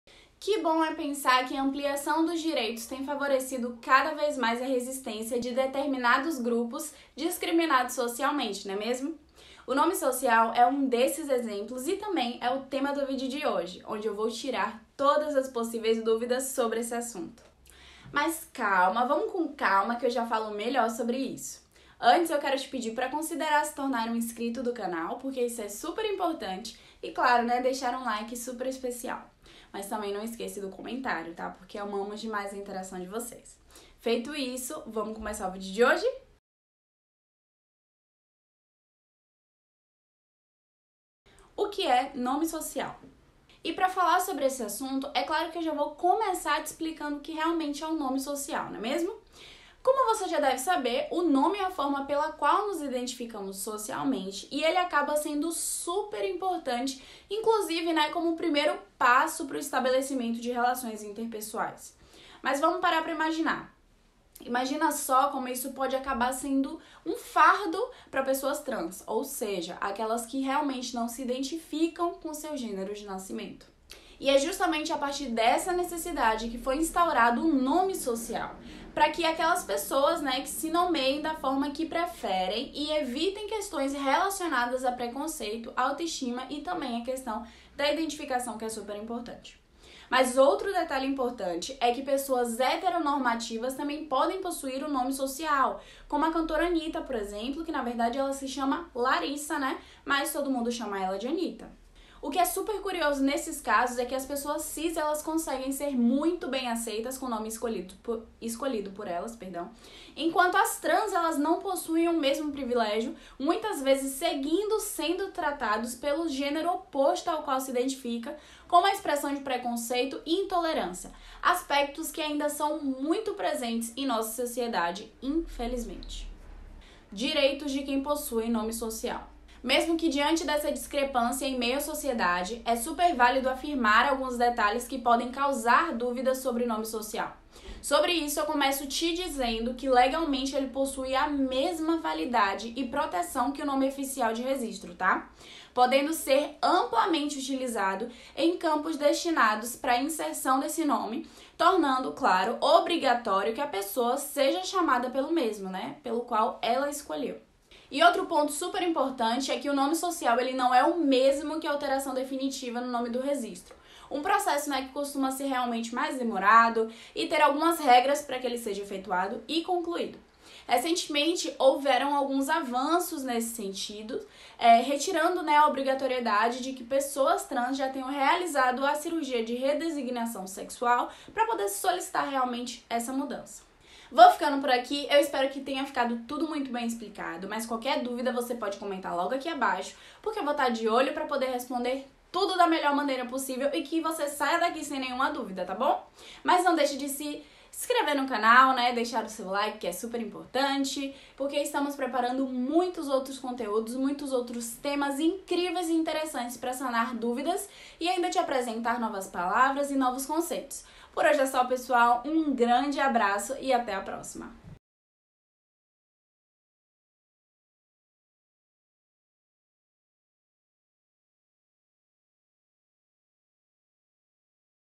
O QUE É NOME SOCIAL?
Que bom é pensar que a ampliação dos direitos tem favorecido cada vez mais a resistência de grupos discriminados socialmente, não é mesmo? O nome social é um desses exemplos e também o tema do vídeo de hoje onde tiro todas as possíveis dúvidas sobre o assunto.

00:00 O que é nome social?
02:07 Direitos de quem possui nome social